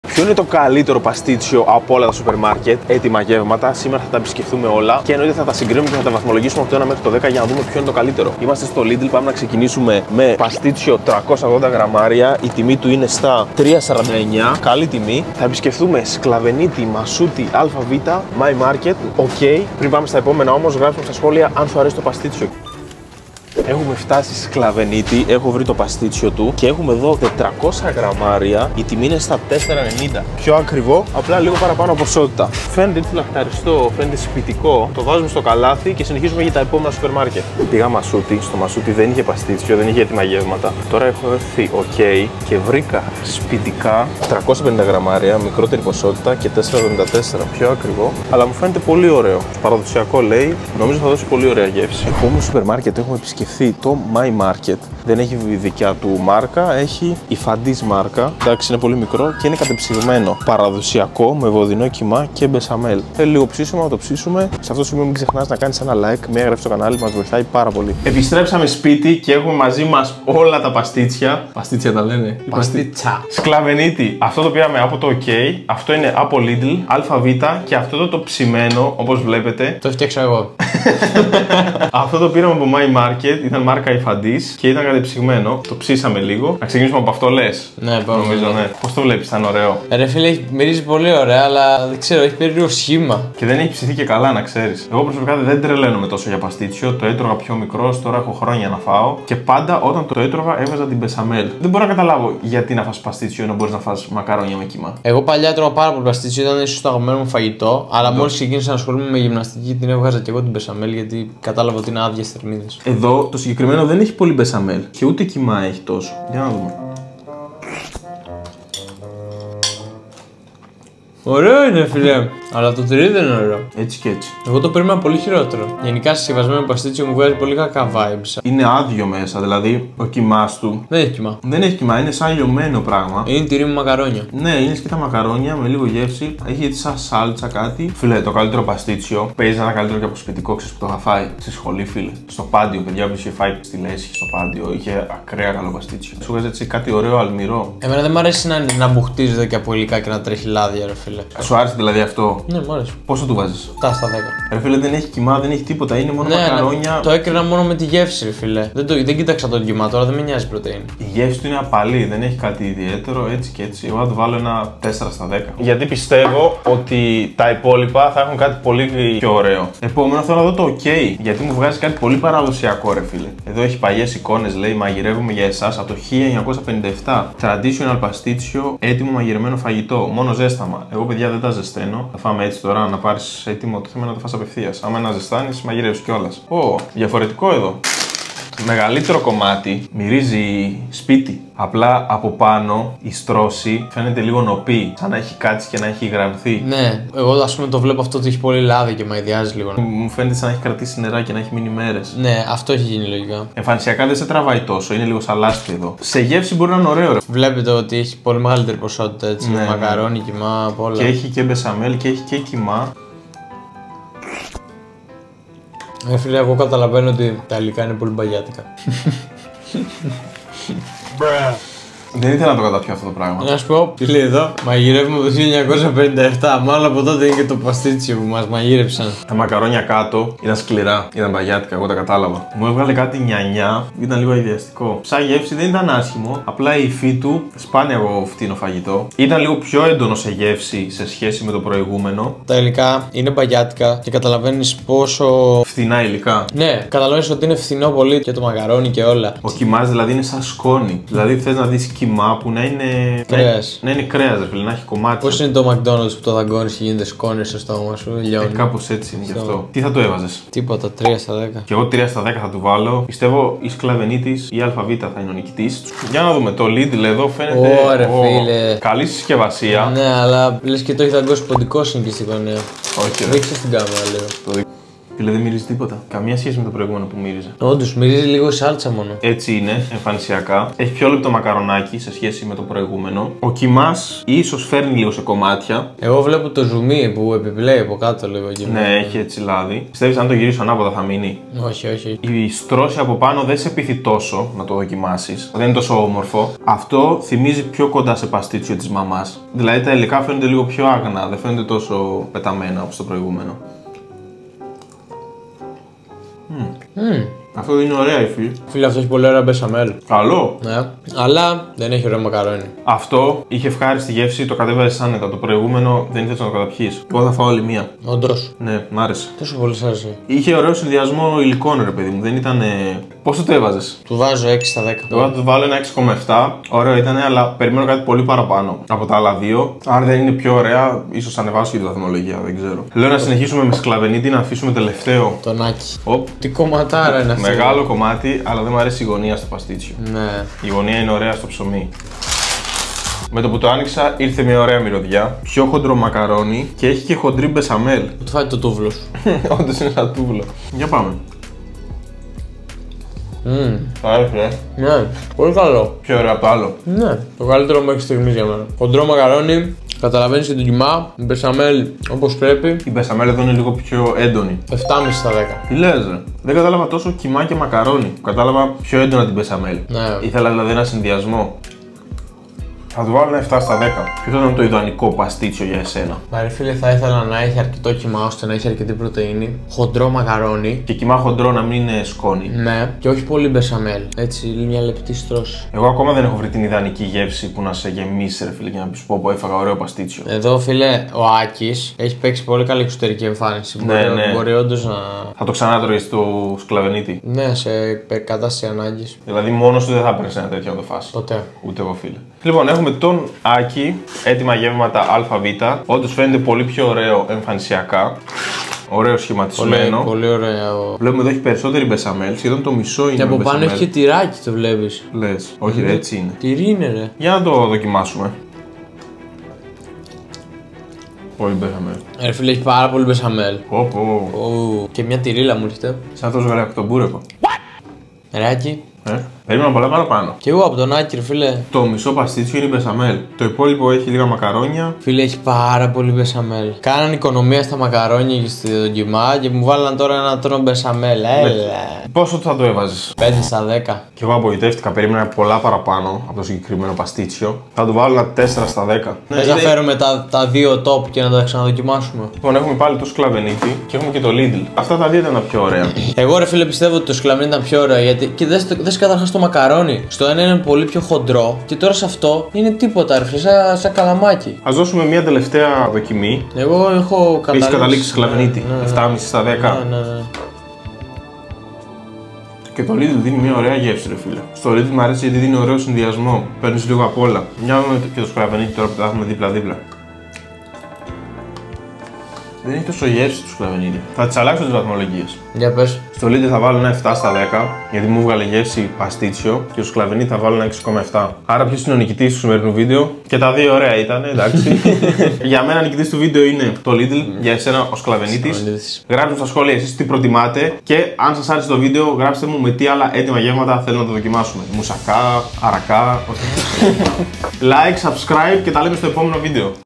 Ποιο Είναι Το Καλύτερο ΠΑΣΤΙΤΣΙΟ Από Σουπερ Μαρκετ;
Ποιο σουπερ μαρκετ (ΑΒ, Σκλαβενίτης , Lidl, mymarket , ok) με έτοιμα γεύματα έχει το καλύτερο παστίτσιο;